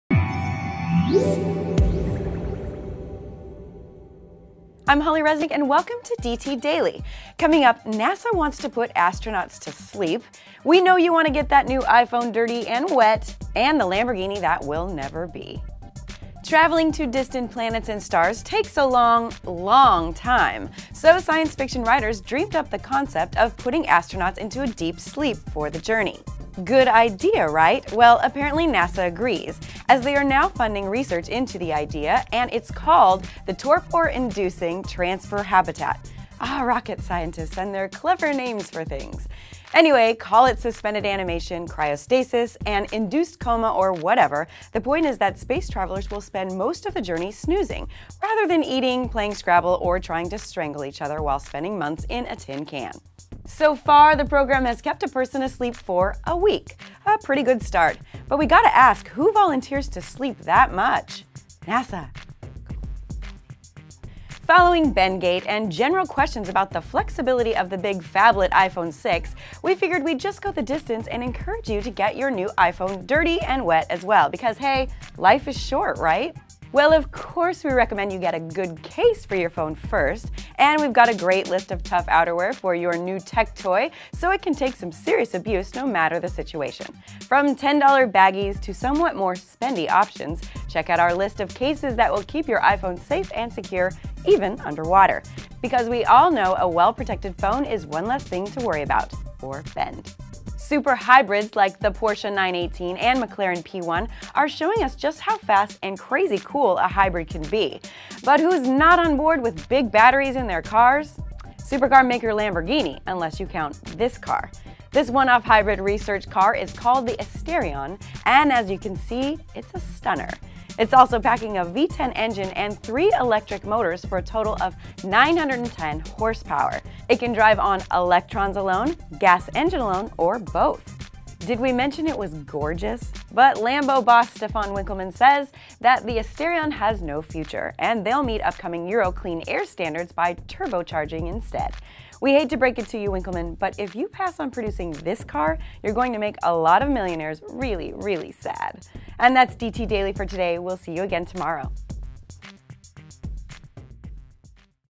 NASA's sleep study, Tough iPhone cases, Lamborghini Hybrid - DT Daily (Oct 8)
Traveling to distant planets and stars takes a long, LONG, time, so long ago, science fiction writers dreamed up the concept of putting astronauts into a deep sleep for the journey.

Good idea, right? Well apparently NASA agrees, as they are now funding research into the idea, and it’s called the Torpor Inducing Transfer Habitat. Ah, rocket scientists and their clever names for things. Anyway, call it suspended animation, cryostasis, an induced coma or whatever, the point is that space travelers will spend most of the journey snoozing, rather than eating, playing scrabble or trying to strangle each other while spending months in a tin can.

So far, the program has kept a person asleep for a week – a pretty good start. But we gotta ask – who volunteers to sleep that much? Besides, everyone…

Following Bendgate and general questions about the flexibility of the big phablet iPhone 6, we figured we’d just go the distance and encourage you to get your new iPhone dirty and wet as well, because hey, life is short, right?

Well of course we recommend you get a good CASE for your phone first, and we’ve got a great list of tough outerwear for your new tech toy so it can take some serious abuse no matter the situation. From $10 baggies to somewhat more spendy options, check out our list of cases that will keep your iPhone safe and secure, even underwater! Because we all know a well-protected phone is one less thing to worry about. Or bend.

Super hybrids like the Porsche 918 and McLaren P1 are showing us just how fast – and crazy cool – a hybrid can be. But who’s not on board with big batteries in their cars? Supercar maker Lamborghini – unless you count the one-off hybrid research car called the Asterion, and it’s a stunner. It’s also packing a V10 engine and three electric motors for a total of 910 horsepower. It can drive on electrons alone, gas engine alone, or both.

Did we mention it was gorgeous? But Lambo boss Stephan Winkelmann says the Asterion has no future, and they’ll meet upcoming Euro clean air standards with turbocharging instead. We hate to break it to you Winklemann, but if you pass on producing the Asterion, you’re going to make a lot of millionaires really, really sad.

Your host today is Holly Resnick.